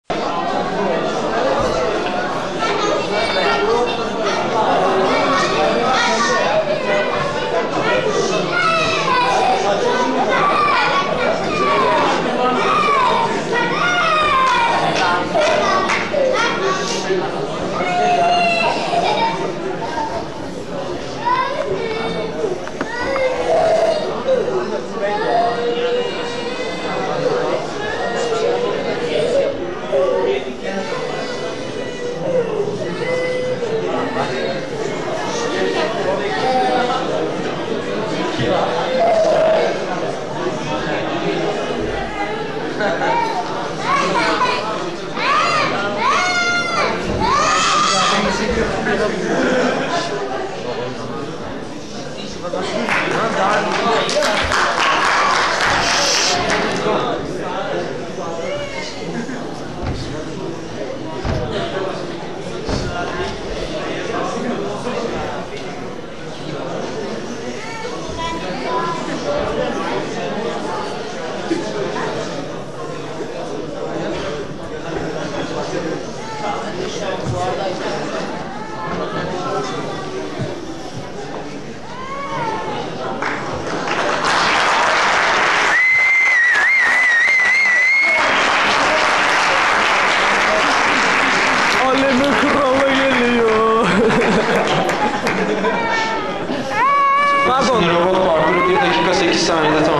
Tıfıl - Slalom Robot - Odtü Robot Günleri 2011
Slalom Robot Kategorisi 3.sü olan TIFIL adlı robot.